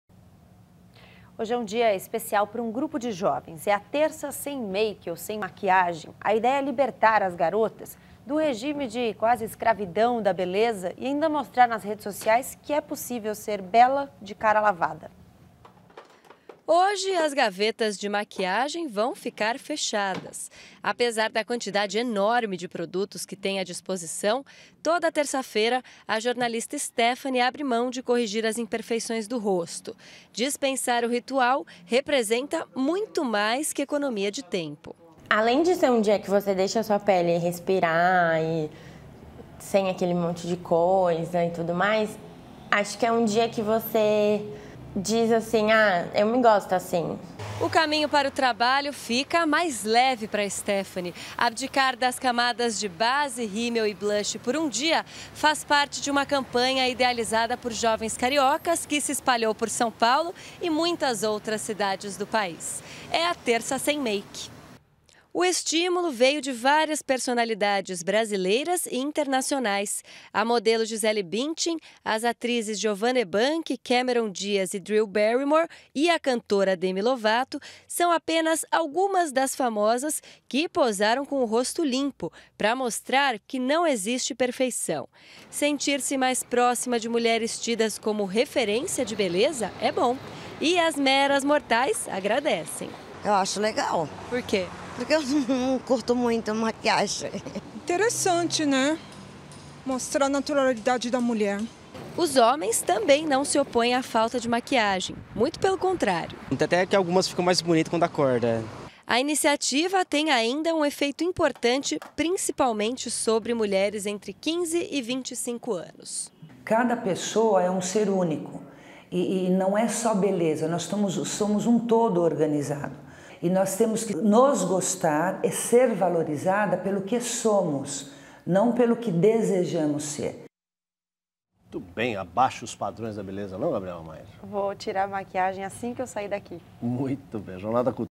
Grupo de jovens criam a "Terça sem Make"
A campanha é um alerta  contra o preconceito.